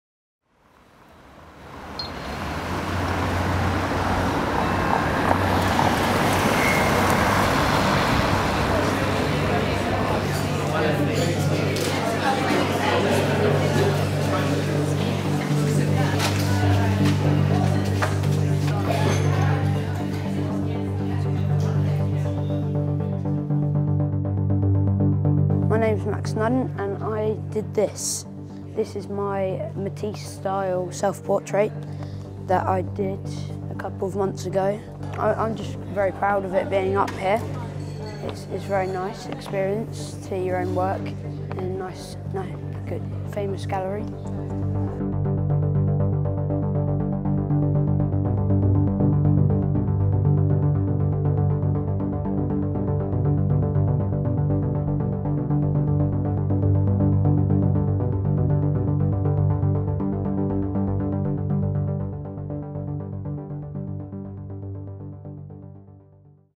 Generation ART: Young Artists on Tour - Max Snudden
Filmed at the exhibition's launch at Turner Contemporary in June 2015, this film celebrates Generation ART: Young Artists on Tour, an exhibition of artwork by children and young people from across England. This film focusses on the work of Max Snudden, who painted a self portrait inspired by the work of Matisse.

Run by engage, the National Association for Gallery Education the project is supported by Arts Council England’s Strategic touring programme, the Garfield Weston Foundation and the Helen Jean Cope Charity. Generation ART will tour to New Walk Museum and Art Gallery and Soft Touch Arts in Leicester, and Quay Arts on the Isle of Wight in 2016.